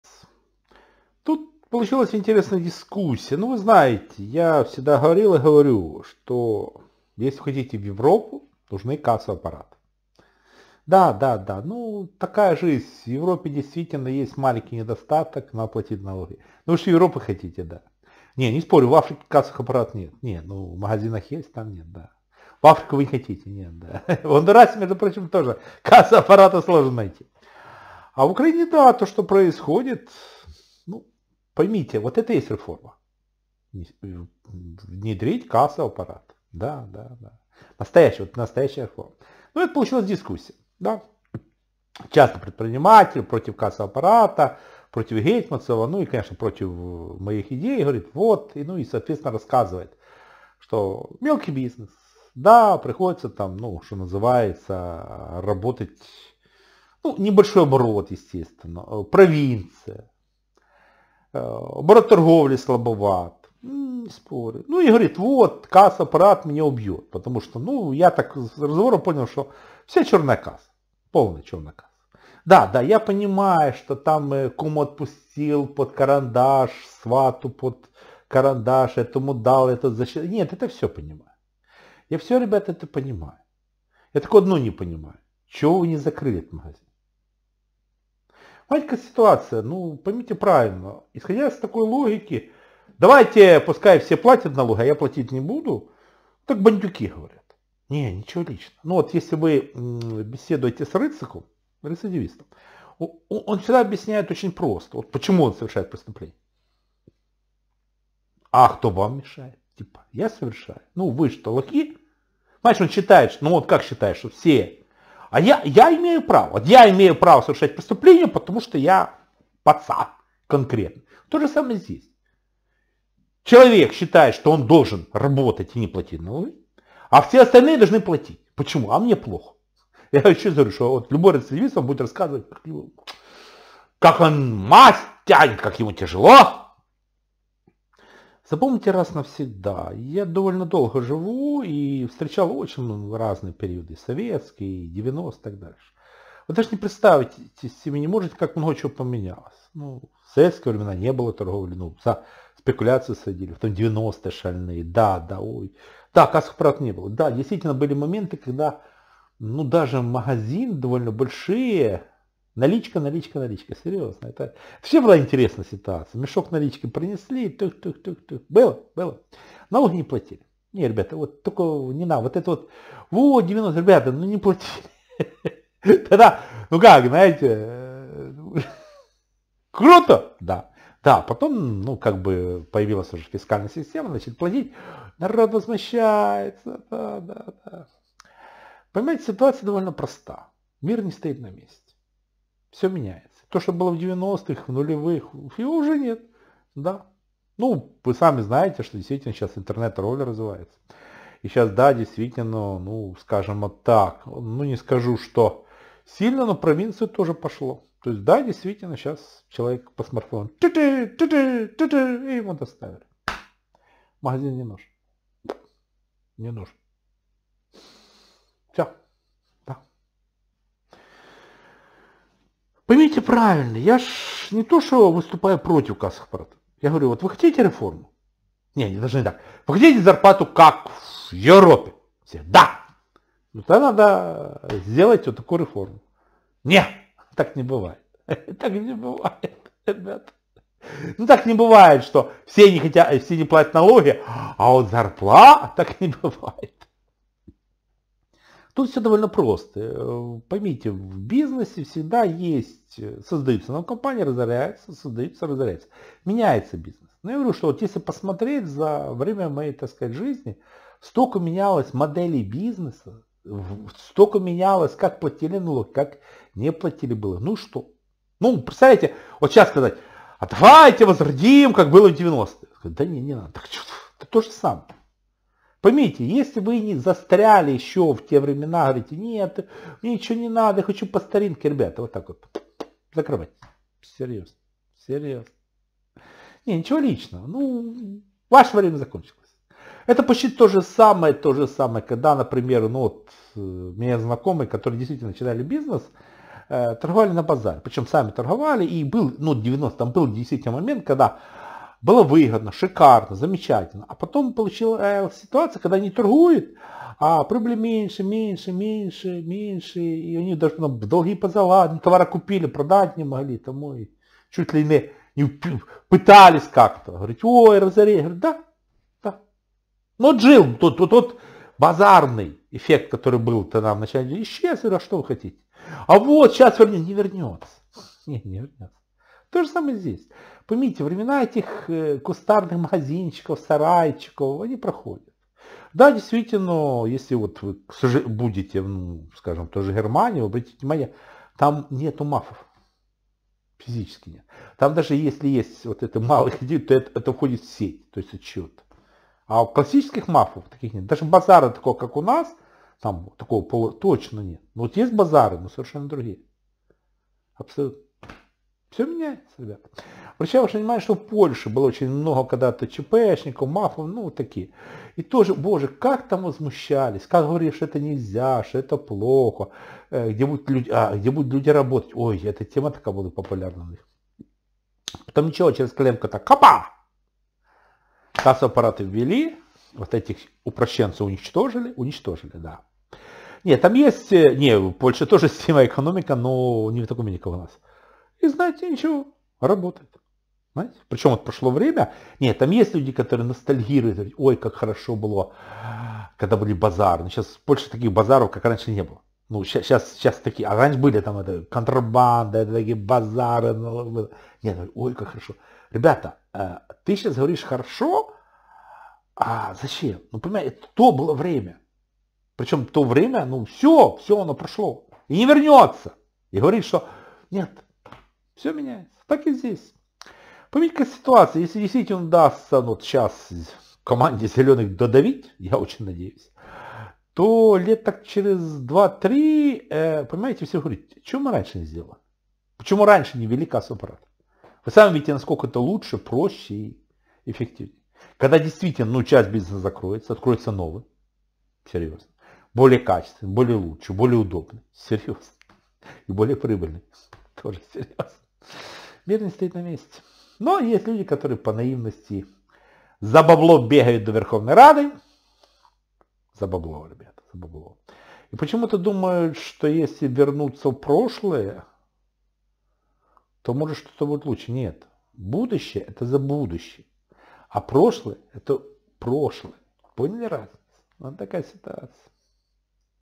Почему некоторые мелкие бизнесмены хотят, чтобы они воровали, но им ничего за это не было
Я хорошо знаю и понимаю, что народ не любит платить налоги. Тем более те кто уже привык работать в упрощенке и лихо не платить налоги. Это приятно и это хорошо, что можно воровать деньги из госбюджета (не уплата налогов это обычное воровство) и при этом требовать от Зеленского, чтобы зарплата была, как  в Европе. Для того, чтобы Ваша зарплата была, как в Европе, в начале научитесь платить налоги, как в Европе